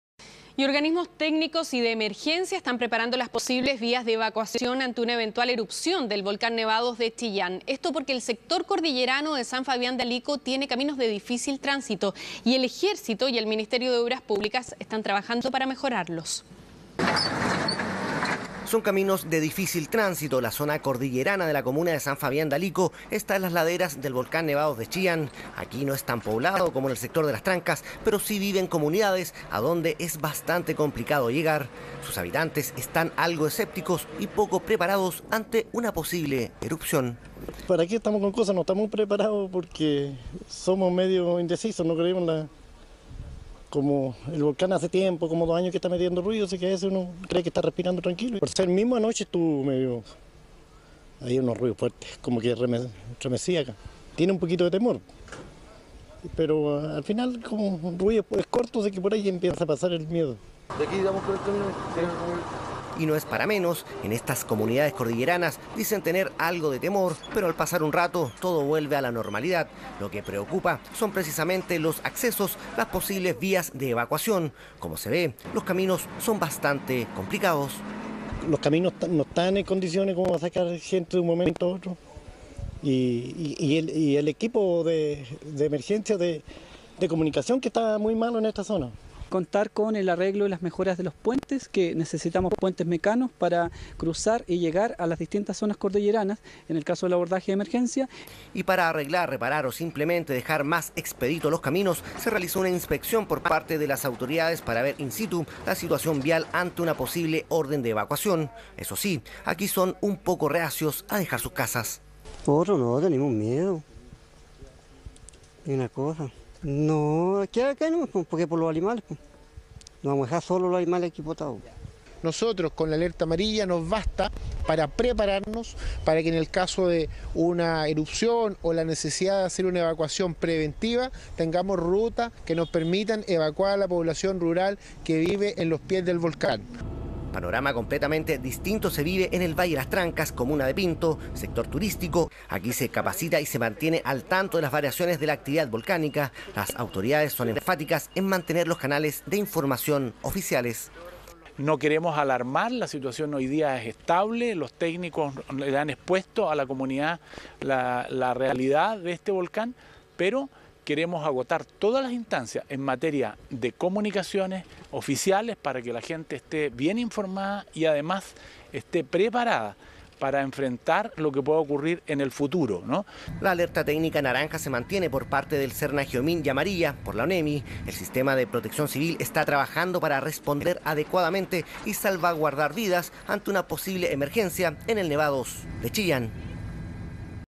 Ministerio de Minería encabezó visita a Nevados de Chillán para revisar situación del volcán.
El secretario de Estado viajó junto a los directores de Onemi y SERNAGEOMIN a la Región del Biobío con el fin de monitorear en terreno el comportamiento del macizo e informar a la comunidad el funcionamiento del Sistema de Protección Civil ante un escenario de mayor riesgo.